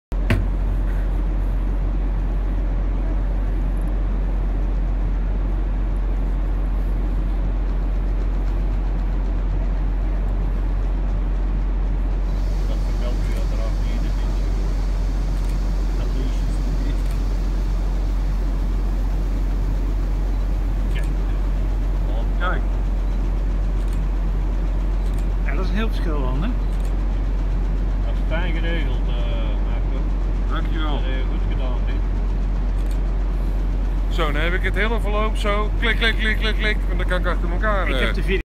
Dust Suppression Hopper Loading Ammonium Sulphate at Europort
At the Europort, Belgium, ammonium sulphate is loaded into a ship's hold via an enclosed conveyor belt with an outlet chute and stainless steel Dust Suppression Hopper.